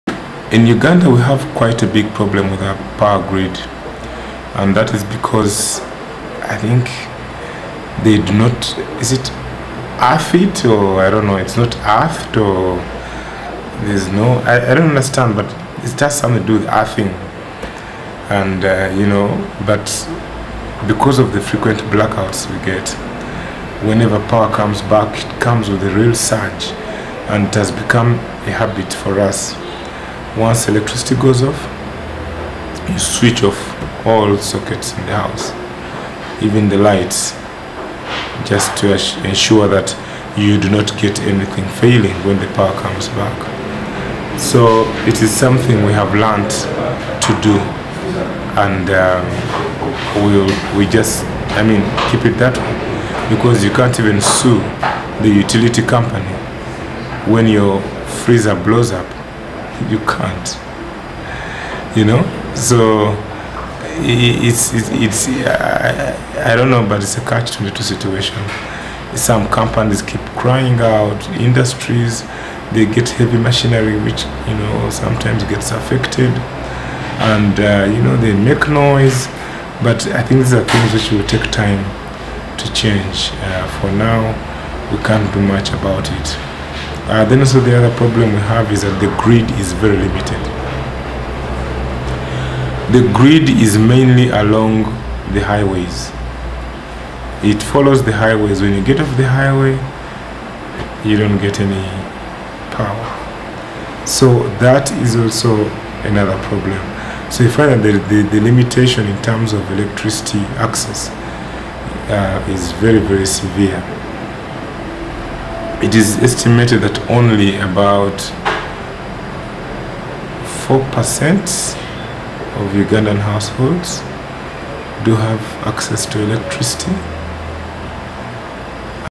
James Wire on the lack of electricity in Uganda
James Wire of LinuxSolutions talks about the challenges of working with electricity in Uganda, and how hard it is to get electricity.